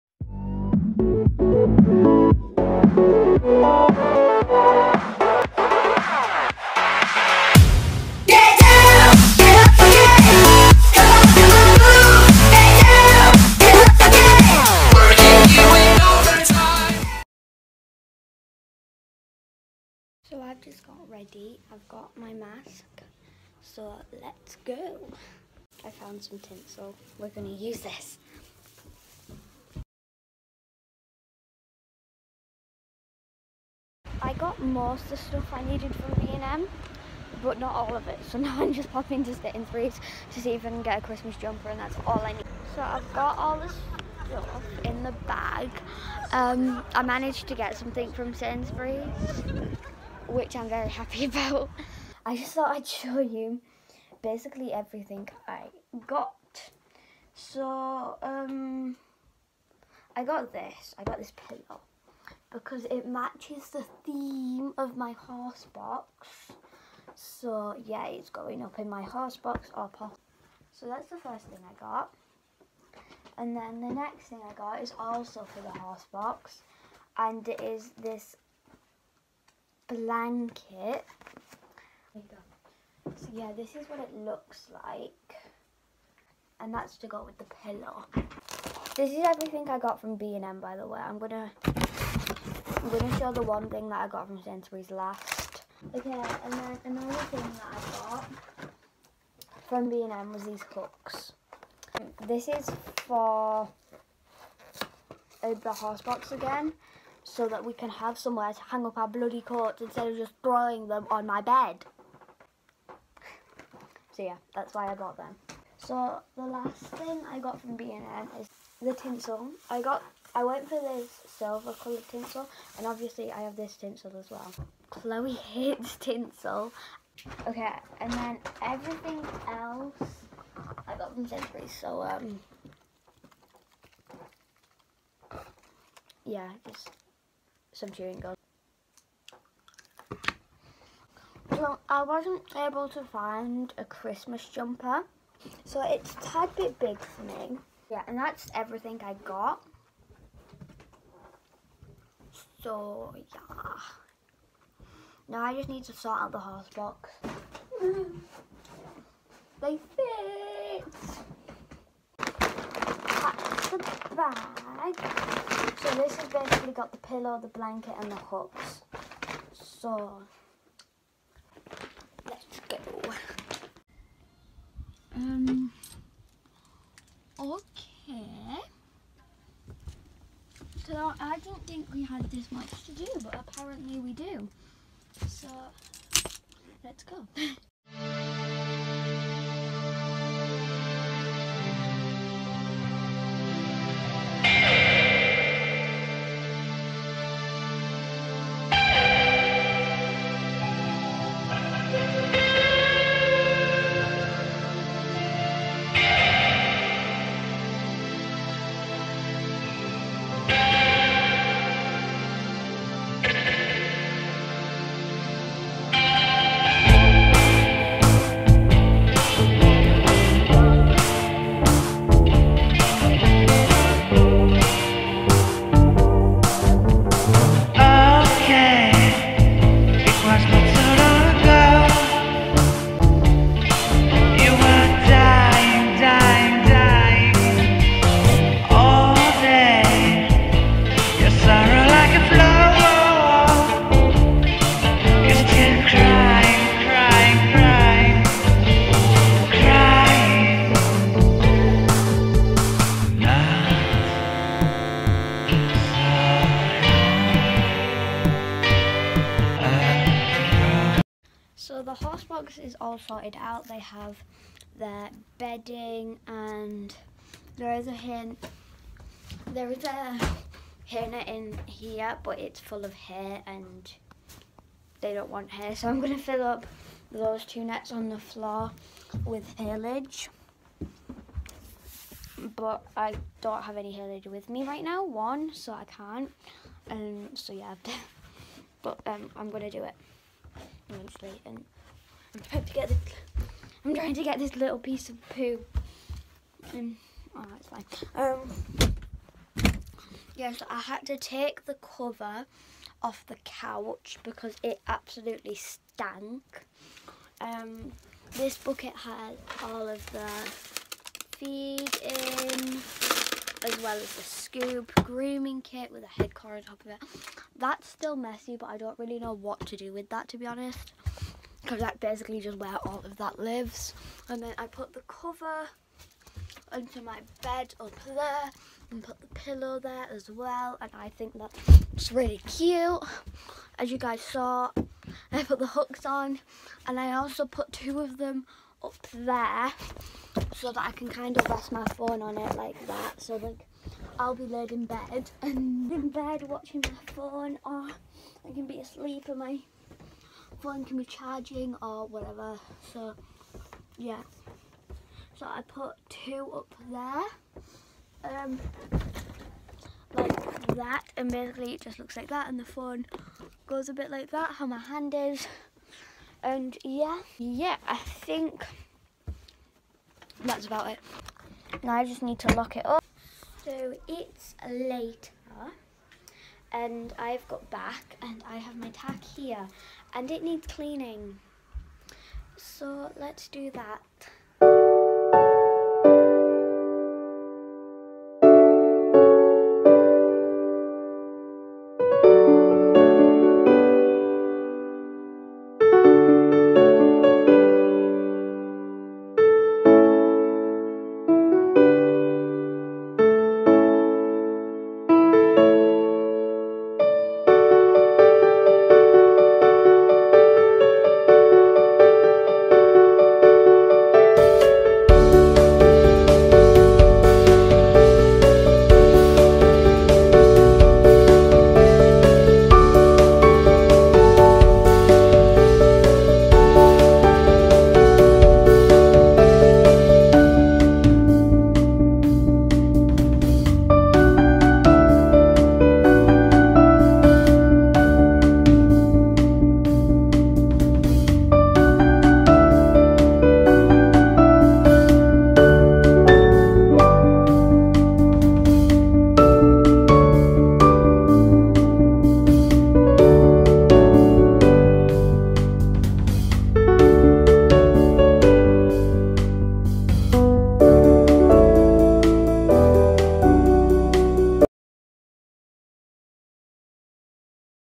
Christmas Show! | get the horse box ready and go shopping with me!!!
Get ready for a show with me 😌

Merry Christmas if your seeing this before Christmas and hope you had an amazing Christmas if your seeing this after

Comment video ideas because i font have many good ideas

Also comment some days that i should post on ideally once a week

Love all of you and have an amazing day 🙂